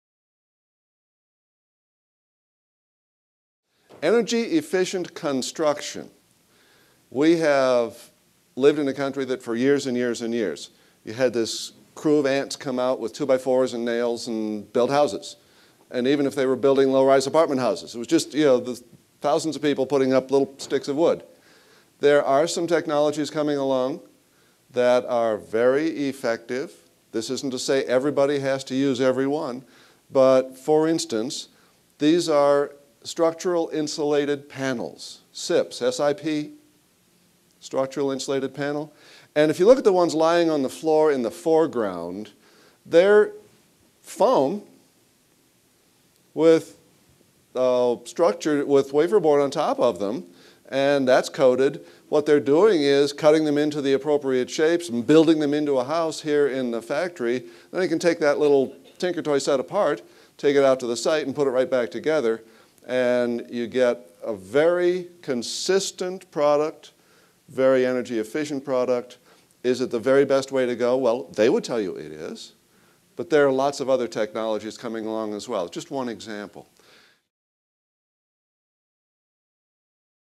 Structural Insulated Panels
Structural Insulated Panels (SIPs) are composite building materials made of two structural boards with rigid foam in between.  These panels can help tighten a home and make it more energy efficient.  Find out more about what they are and how they can help reduce energy costs in this clip from EcoBroker's 2008 Green Real Estate Conference.